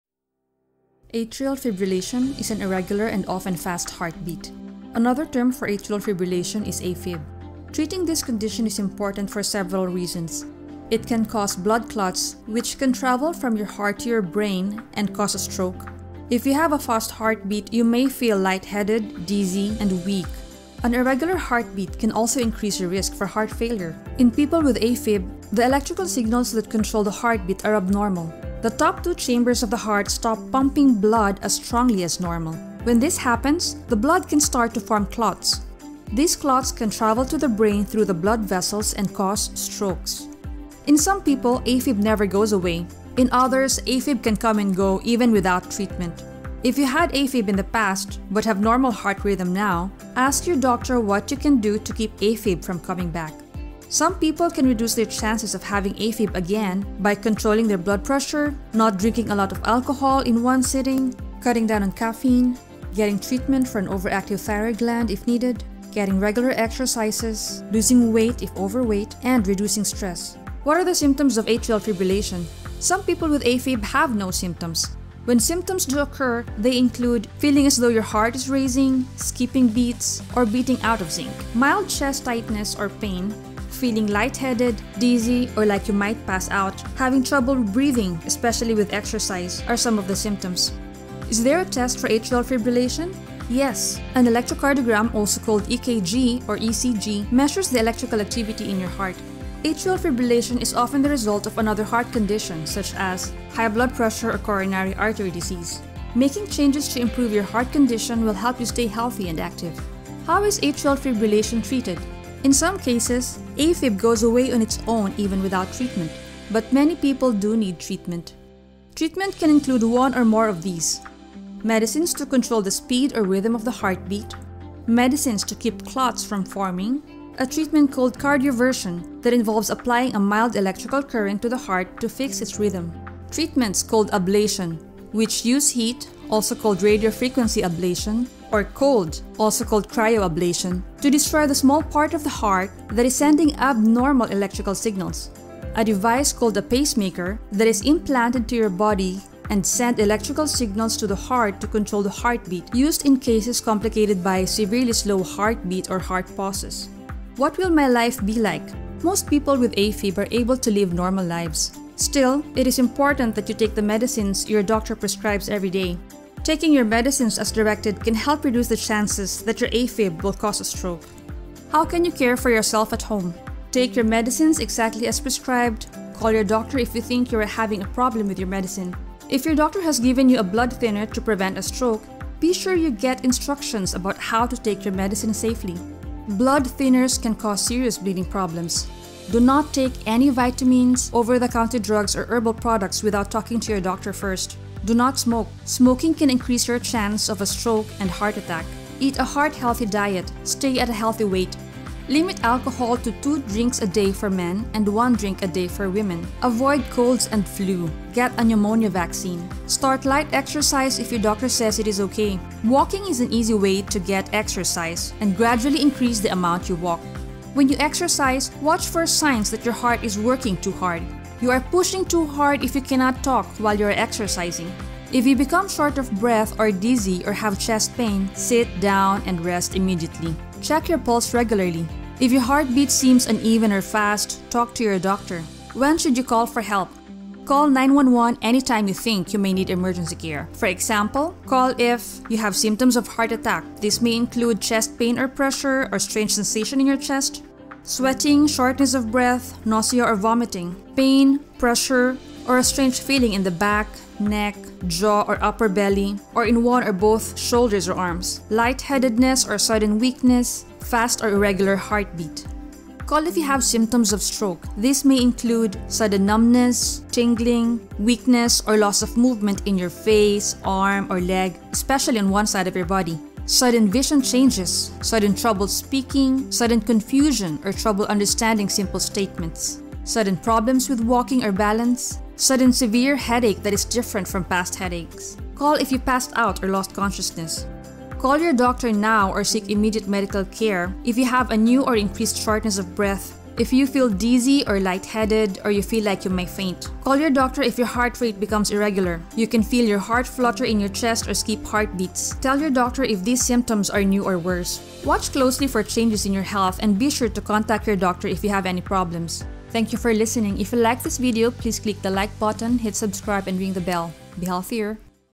Atrial Fibrillation
This is an educational video on a heart disease called atrial fibrillation, also called "a fib", which is characterized by irregular and often fast heartbeat.  A Fib is diagnosed with ECG if it occurs frequently, sometimes a holter monitor or cardiac monitor is needed to capture it.
Source:
UptoDate2019